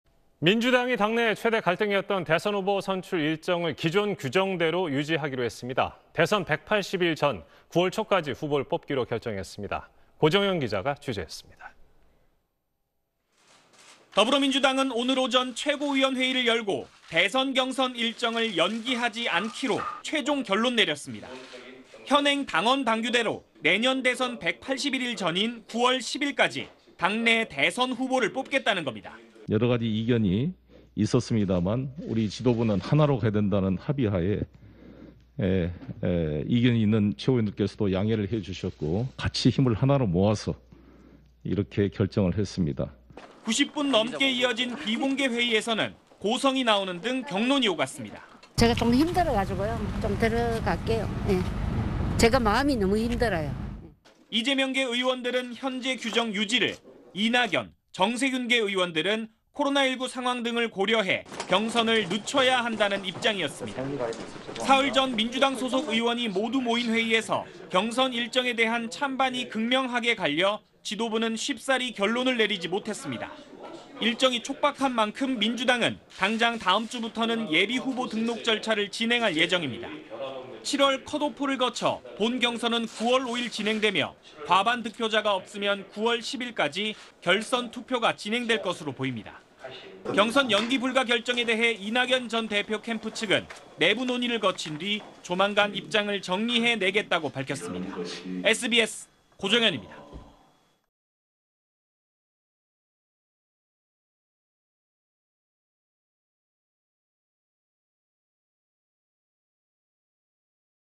민주당, 경선 일정 규정대로…9월 초 당 대선 후보 선출 / SBS
민주당이 당내 최대 갈등 사안이었던 대선후보 선출 일정을 기존 규정대로 유지하기로 했습니다. 대선 180일 전, 9월 초까지 후보를 뽑기로 결정했습니다.
고정현 기자가 취재했습니다.